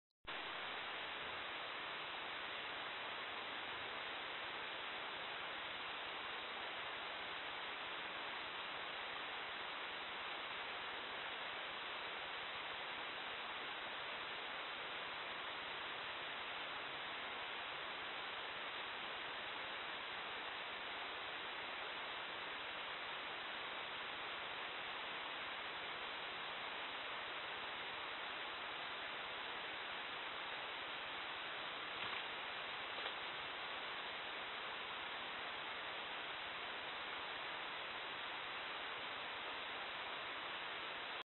Venado en Redes
Vídeo de venado tomado con una cámara nocturna en el Parque Natural de Redes